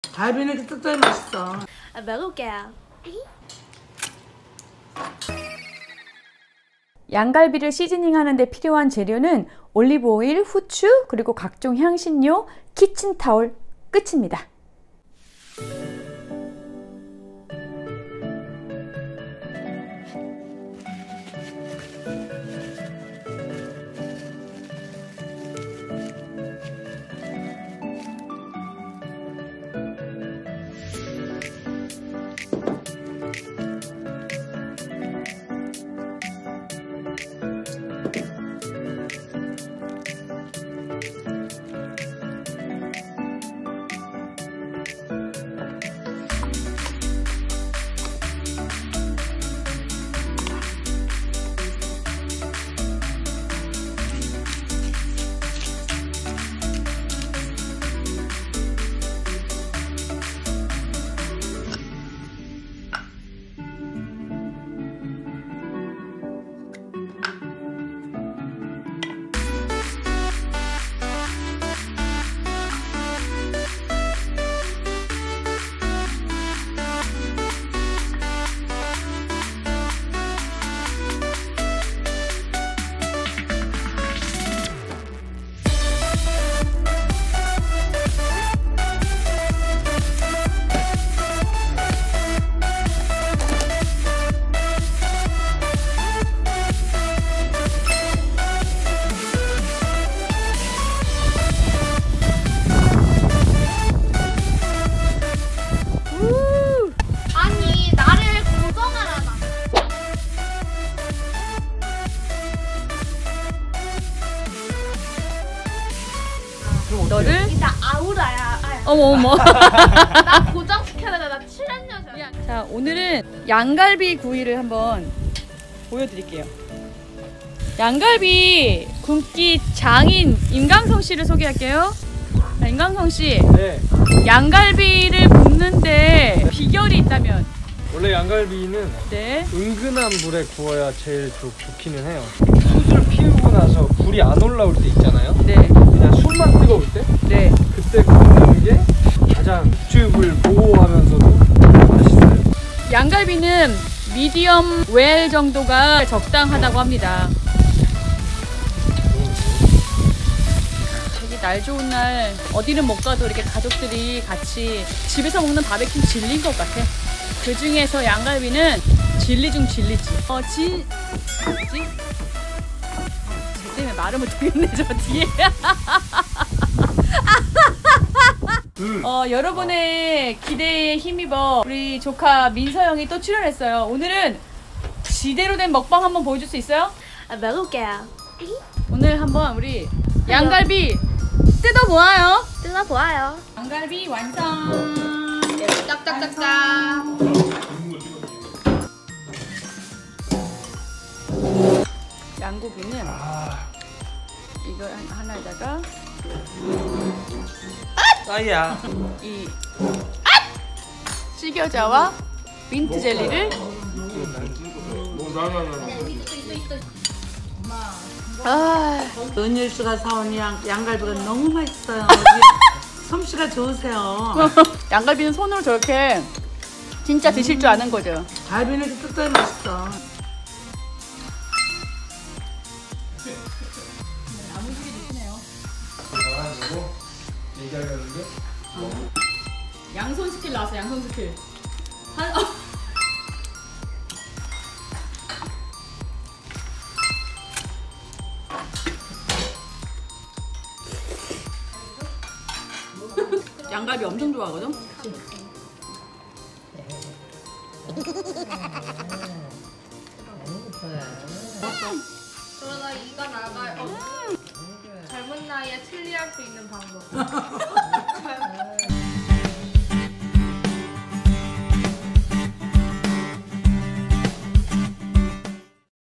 캠핑요리 추천 양갈비 숯불구이! 인생 최고의 양갈비 스테이크 만드는 법부터 굽고 먹방까지(feat.바베큐장인 임강성)
양갈비 집에서 해먹는 게 어렵다고 생각되시는 분들이 많더라구요.
제가 사용한 향신료가 다 없다면 후추, 건 로즈마리, 건 오레가노 세가지로 도전해보세요.
캠핑 가서 숯불에 양갈비 바베큐 드셔보시면 더이상 삼겹살 생각 안나실꺼에요
돼지고기, 소고기 다 잊혀집니다~
저는 집에서 오븐에 구워도 냄새 하나도 안나고 맛있어요.
제 영상 중 양갈비 스테이크 편 참고

양갈비 스테이크는 씨겨자와 민트젤리를 함께 드시는 것을 추천해요!